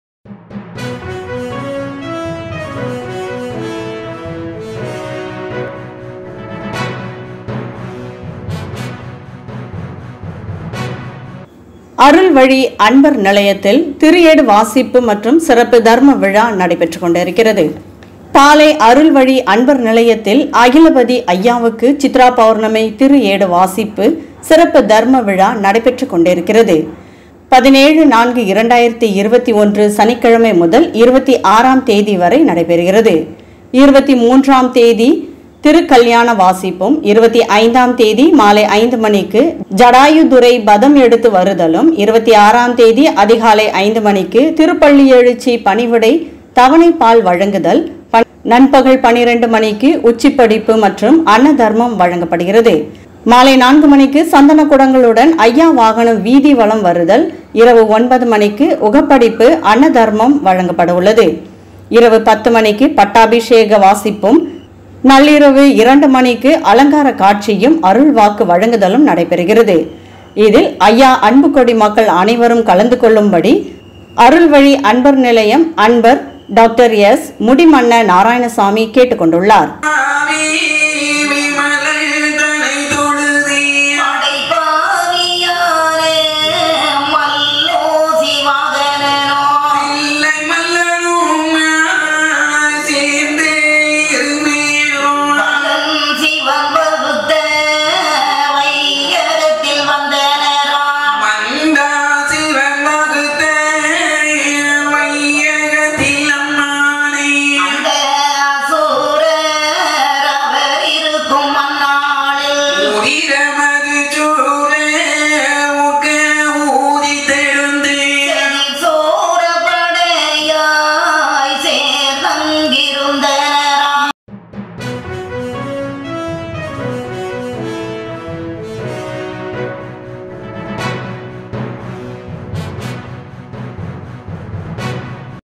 அருள்வழி அன்பர் நிலையத்தில் திருஏடு வாசிப்பு
பாளை அருள்வழி  அன்பர் நிலையத்தில் அகில் அப்படி அய்யாவுக்கு சித்ராபௌர்ணமி திருஏடு வாசிப்பு சிறப்பு விழா நடைபெறுகிறது